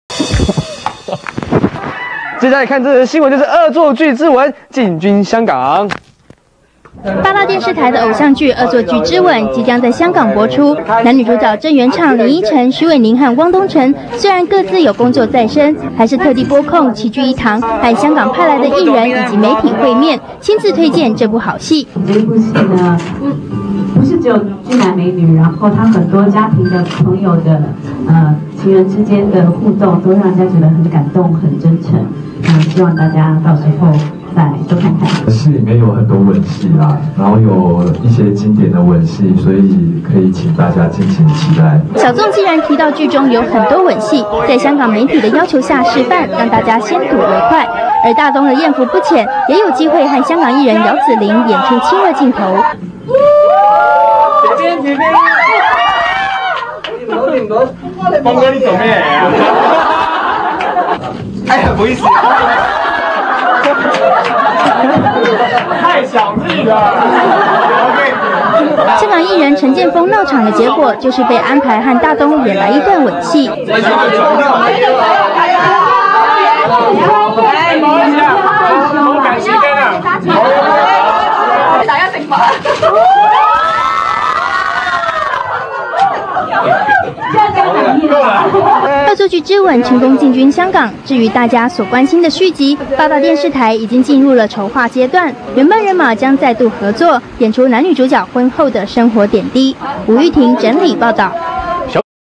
5/30/06 New - ISWAK TVB Promotion
Finally...the news of the promotion they had for TVB. Seems like everyone was having fun. Joe and Ariel looks like they are comfortable with each other, but too bad they didn't kiss.